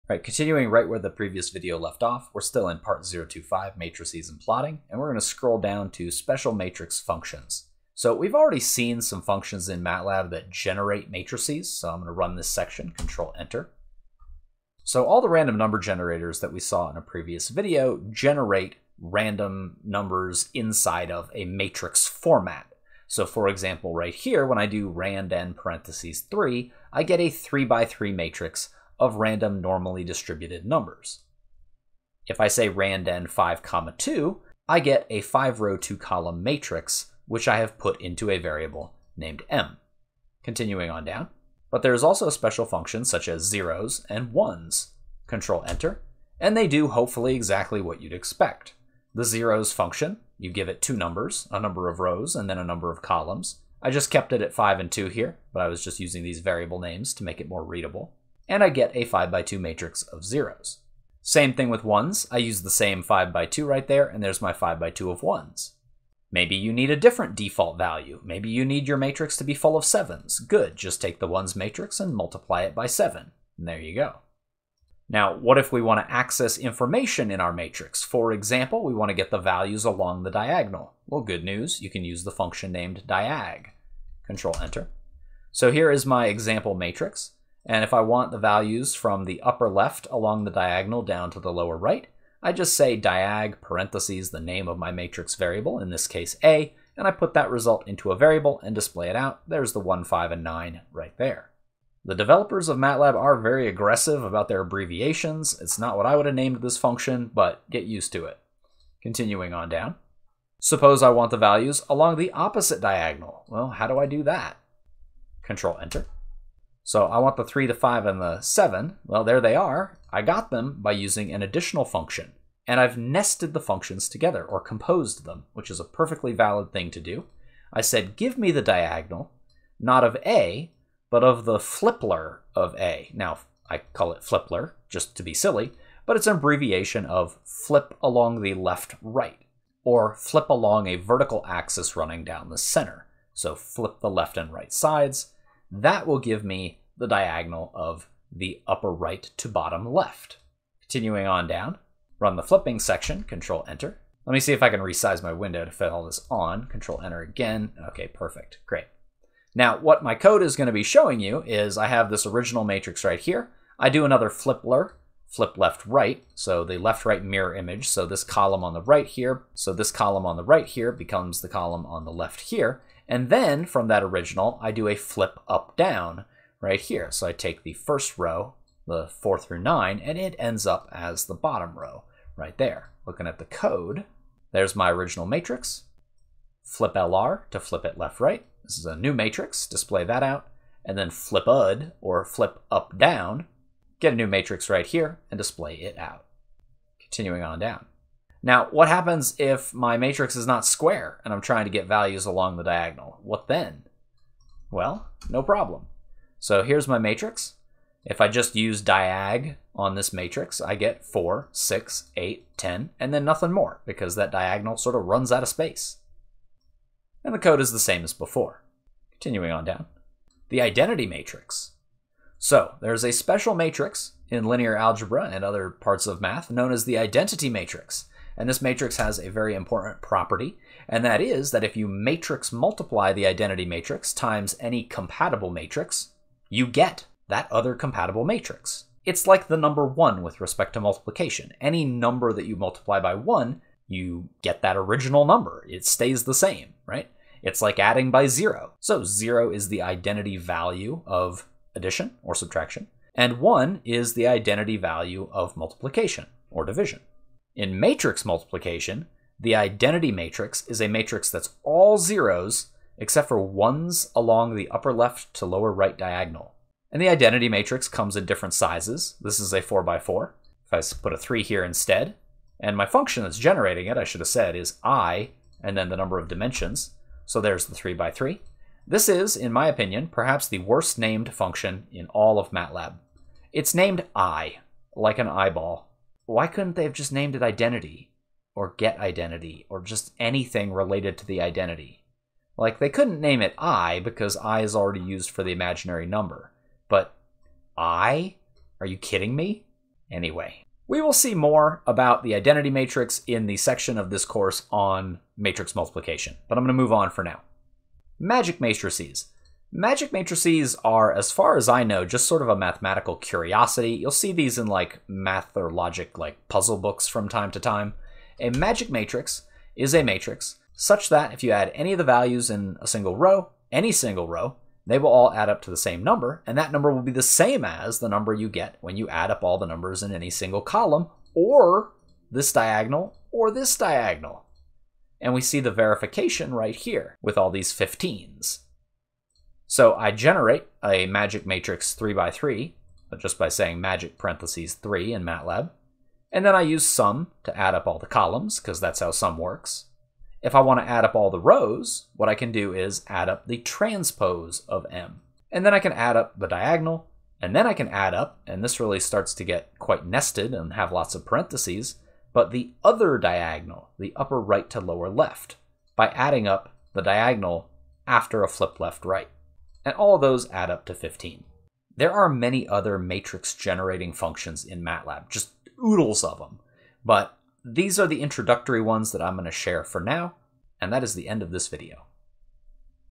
Matlab: Special Matrix Functions
This video demonstrates special matrix functions for generating matrices with a uniform value, the identity matrix, magic matrices, as well as functions for flipping a matrix across different axes or accessing the values along the diagonal in Matlab.

All the code except for one alternative line (discussed in the video) works perfectly in Octave.

This video is part of a series comprising a complete introductory course in Matlab / Octave.

A direct link to the code used in this video is